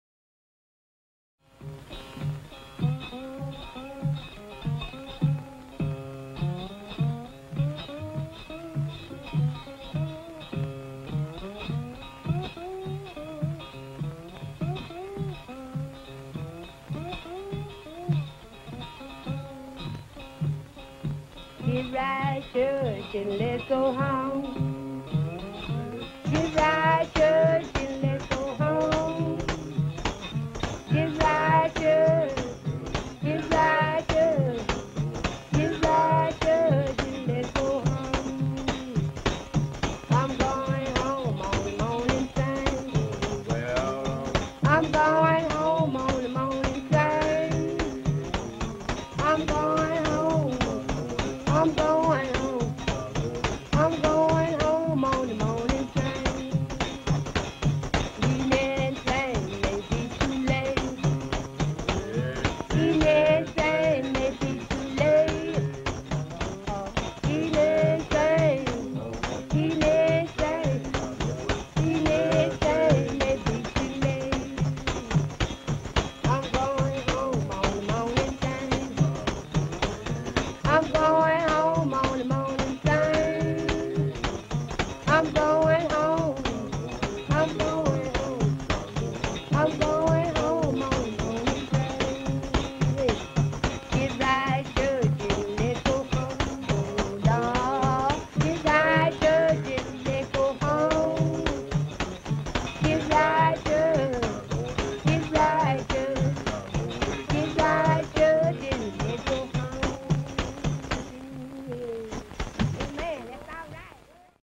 Diddley Bow Song
A video of a group of African Americans performing a song on and singing along to/accompanying a diddley bow. The diddley bow is likely related to various one string, West African instruments, used especially in traditional Ghanaian music. Those instruments include the gonjey (A type of gourd-fiddle), the kolgo, and the jenjeli (An instrument related to the Brazilian berimbau). These instruments all likely descend from the mouth bow. This footage was recorded by Alan Lomax and originally uploaded by GtrWrkShp.